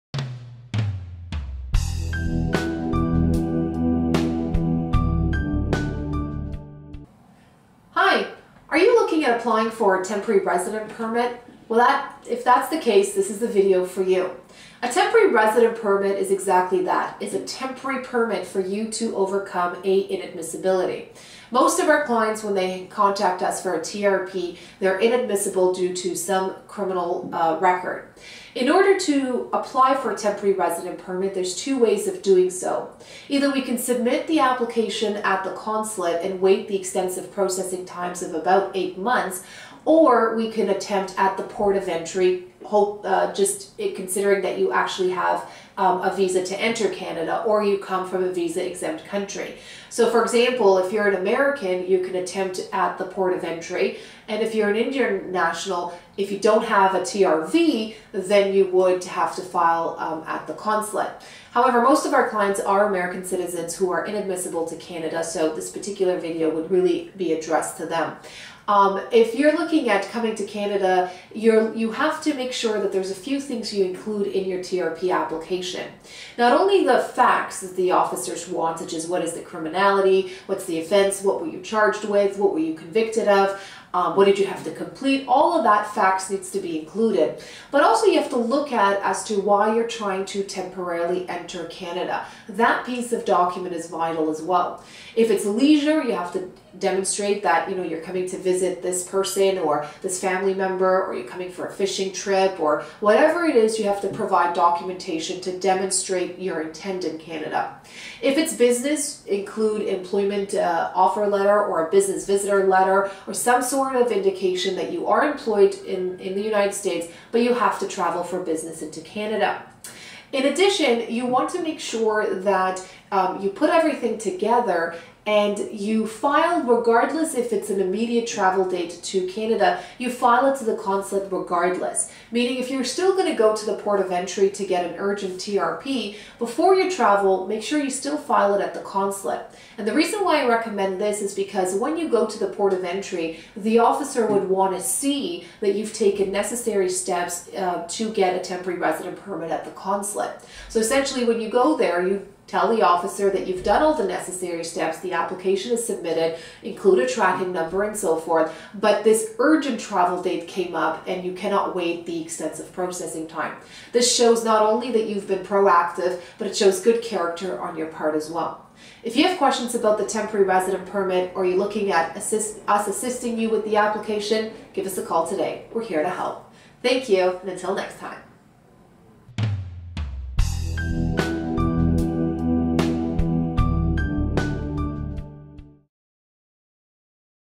Temporary Resident Permit Overcoming Inadmissibility
Have you been denied entry to Canada and have an urgent travel date? A Temporary Resident Permit (TRP) might be your solution to overcome inadmissibility and gain entry into Canada. But should you apply for the TRP at the Port of Entry (POE) or through a consulate?

In this video, we’ll walk you through the process of obtaining a TRP, including the different application methods and the steps involved. We’ll help you understand when and where to file your application to maximize your chances of success.

If you’re facing a travel emergency and need to address your inadmissibility, this video provides crucial information on navigating the TRP application process effectively.

We hope this video assists you in securing your TRP and making your travel plans to Canada.

For more detailed information on Temporary Resident Permits and other immigration matters, visit our websites: